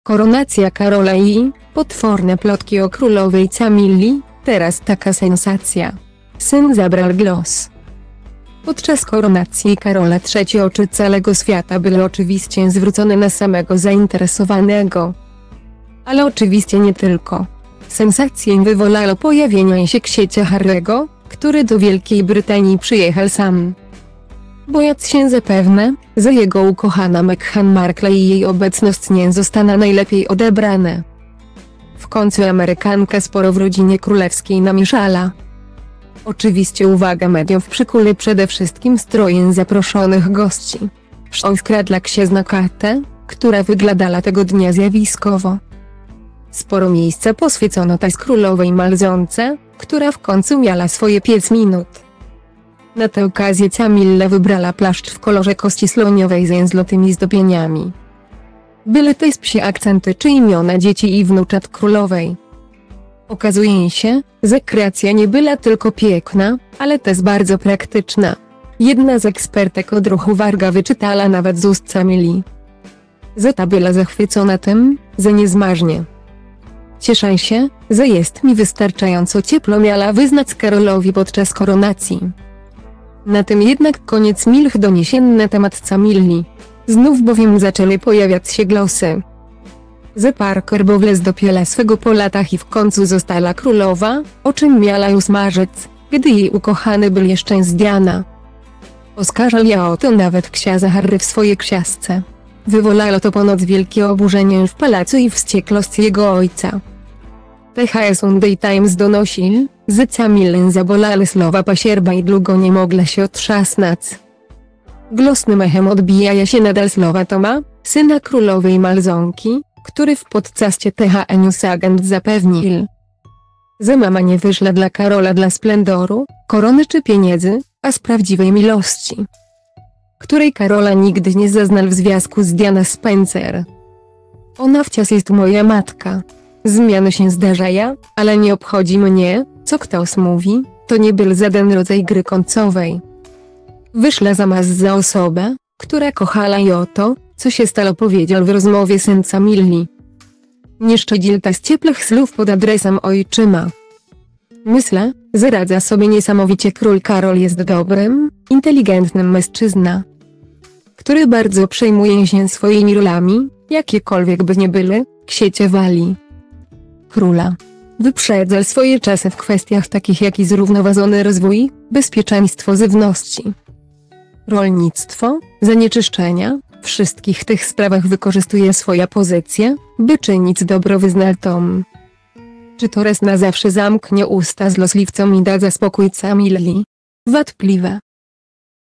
Koronacja Karola III: Potworne plotki o królowej Camilli, teraz taka sensacja! Syn zabral glos
Podczas koronacji Karola III oczy calego swiata byly oczywiscie zwrócone na samego zainteresowanego,ale oczywiscie nie tylko. Sensacje wywolalo pojawienie sie ksiecia Harry'ego, który do Wielkiej Brytanii przyjechal sam,bojac sie zapewne, ze jego ukochana Meghan Markle i jej obecnosc nie zostana najlepiej odebrane.